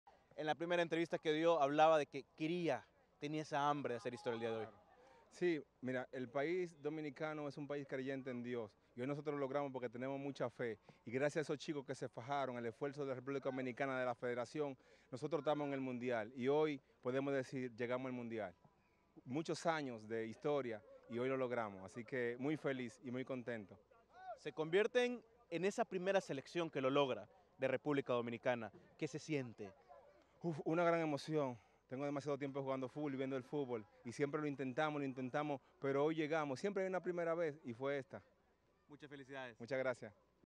Campeonato Sub-20 2022 | Entrevista | Mariano Pérez entrenador de República Dominicana
🇩🇴 🎙️ "Muchos años de historia y hoy lo logramos." - Mariano Pérez, DT de República Dominicana, agradecido con el esfuerzo de sus jugadores y la Federación de República Dominicana.